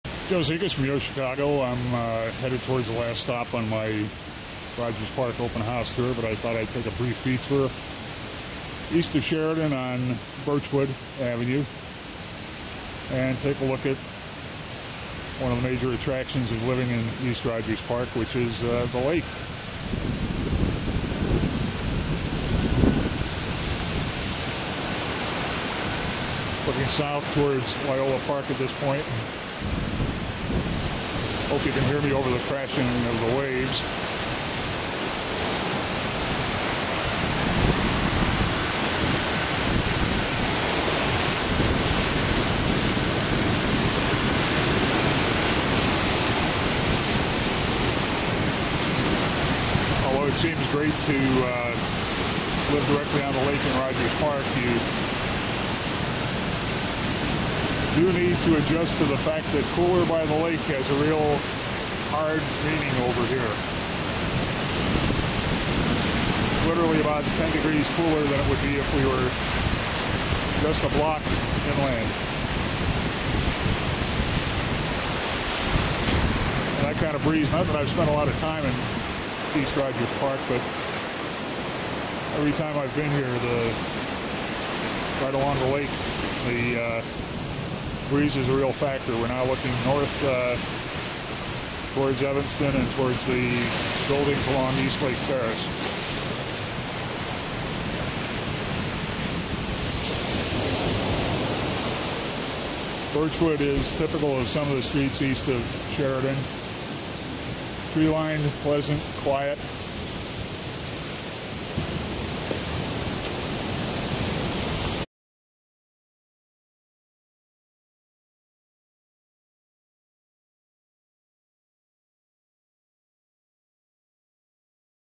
Rogers Park, at the lakefront
I spent Saturday afternoon visiting real estate open houses, but took a brief interlude to scan the lakefront.